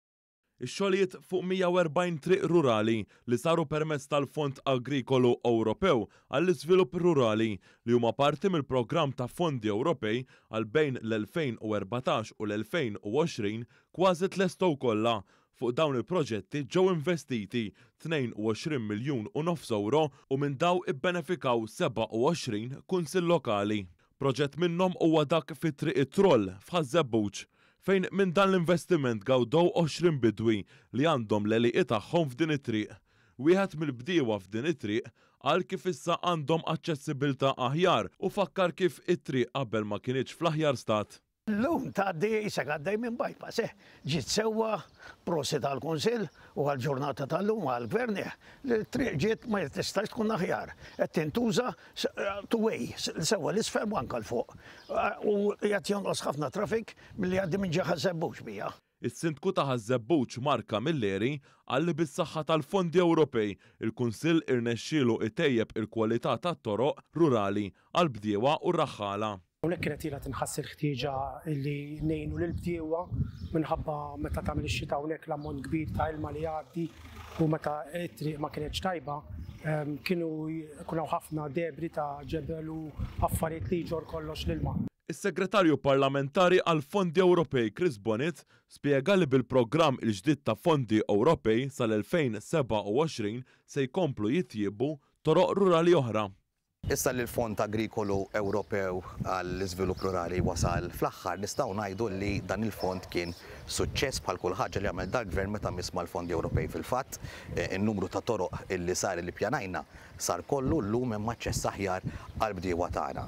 JITLESTEW KWAŻI X-XOGĦLIJIET KOLLHA FUQ 140 TRIQ RURALI
ONE.COM.MT •

Huma mija u erbgħin it-toroq rurali li tlestew, jew kważi tlestew, permezz ta’ fondi Ewropej. Insiru nafu aktar fir-rapport li jmiss.

Ix-xogħlijiet fuq mija u erbgħin triq rurali, li saru permezz tal-Fond Agrikolu Ewropew għall-Iżvilupp Rurali, li huma parti mill-programm ta’ Fondi Ewropew għal bejn l-elfejn u erbatax u l-elfejn u għoxrin, kważi tlestew kollha.

Fuq dawn il-proġetti ġew investiti tnejn u għoxrin miljun u nofs ewro u minn dawn ibbenefikaw sebgħa u għoxrin kunsill lokali.

Proġett minnhom huwa dak fi Triq it-Troll, f’Ħaż-Żebbuġ, fejn minn dan l-investiment gawdew għoxrin bidwi li għandhom l-għelieqi tagħhom f’din it-triq.

Wieħed mill-bdiewa f’din it-triq qal kif issa għandhom aċċessibilità aħjar, u ftakar f’kif it-triq qabel ma kinitx fl-aħjar stat.

JOE MICALLEF
BIDWI
“Illum qisek għaddej minn bypass, ġiet sewwa. Proset għall-Kunsill u l-Gvern tal-ġurnata, li t-triq ma tistax tintuża aħjar. Qegħda two-way, ’l isfel u anki ’l fuq, u qed jonqos it-traffic minn Ħaż-Żebbuġ biha.”

Is-Sindku ta’ Ħaż-Żebbuġ, Mark Camilleri, qal li bis-saħħa tal-Fondi Ewropej, il-Kunsill irnexxilu jtejjeb il-kwalità tat-toroq rurali għal bdiewa u r-raħħala.

MARK CAMILLERI
IS-SINDKU TA’ ĦAŻ-ŻEBBUĠ
“Kienet ilha tinħass il-ħtieġa illi ngħinu lill-bdiewa, minħabba li meta tagħmel ix-xita hawnhekk l-ammont ta’ ilma kbir li jgħaddi... u meta t-triq ma kinitx tajba kien ikun ħafna debris ta’ ġebel u affarijiet oħra li jġorr miegħu l-ilma.”

Is-Segretarju Parlamentari għall-Fondi Ewropej, Chris Bonett, spjega li, bil-programm il-ġdid ta’ fondi Ewropej sal-elfejn sebgħa u għoxrin (2027), se jkomplu jittejbu toroq rurali oħra.

CHRIS BONETT
SEGRETARJU PARLAMENTARI
“Issa li l-Fond Agrikolu Ewropew għall-Iżvilupp wasal fl-aħħar nistgħu ngħidu dan il-fond kien suċċess, bħal kull ħaġa li għamel dan il-Gvern meta mess mal-Fondi Ewropej. Fil-fatt, in-numru ta’ toroq illi ppjanajna sar kollu u llum hemm aċċess aħjar għall-bdiewa tagħna.”